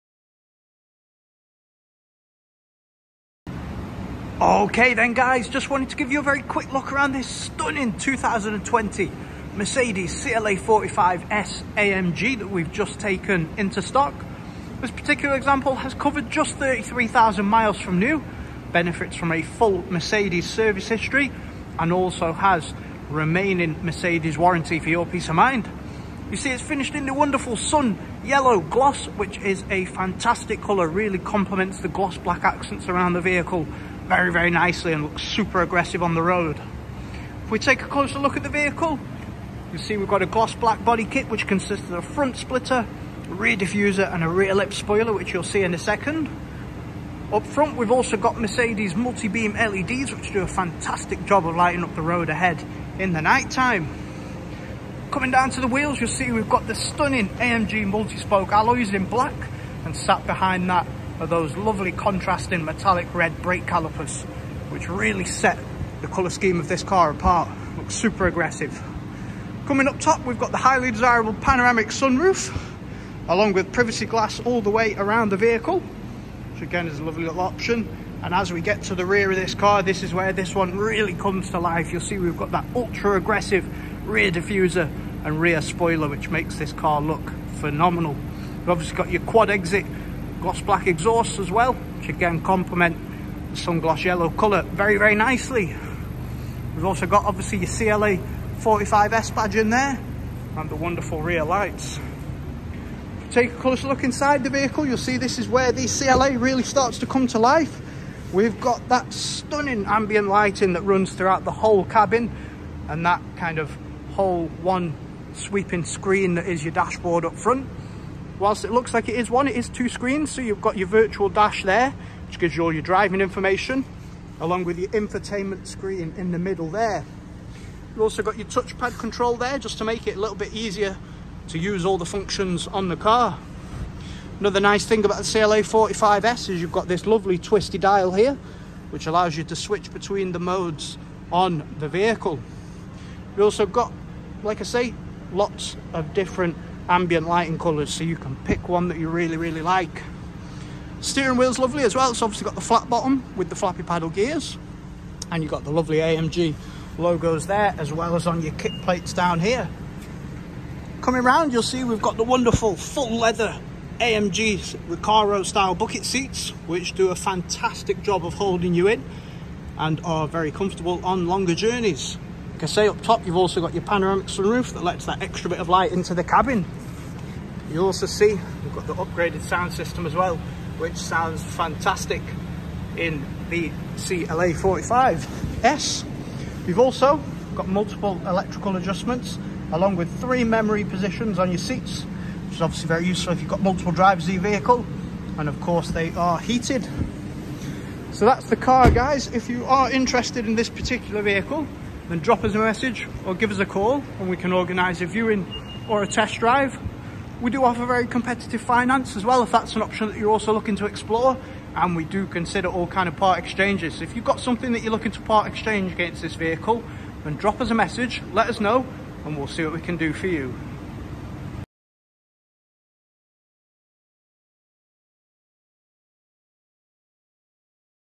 2020 Mercedes CLA45s AMG | Ash Motors Ltd
Take a quick look around this stunning 2020 Mercedes CLA45s AMG we've just taken into stock. For further details on this vehicle, please visit our

200+ 5 Star Google Reviews | Competitive Finance Available | Leeds, Yorkshire